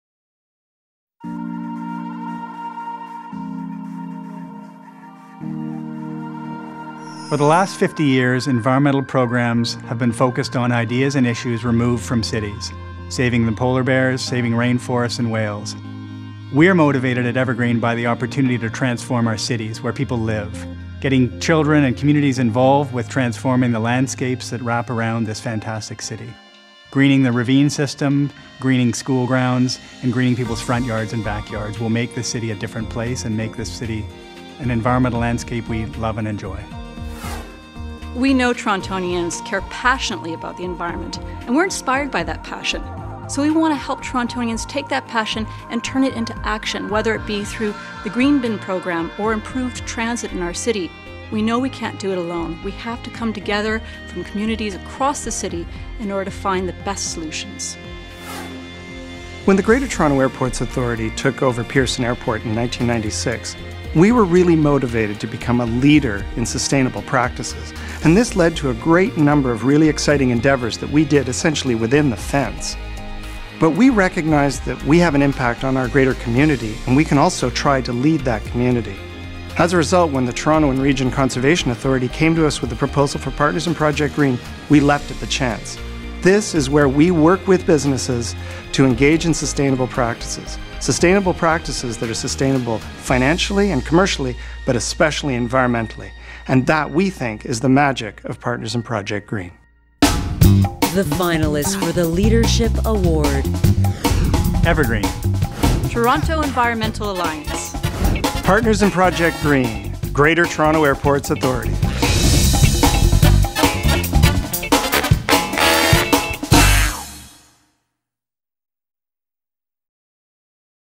Leadership - 2010 Green Toronto Awards
See what inspired our three finalists in the Leadership award category.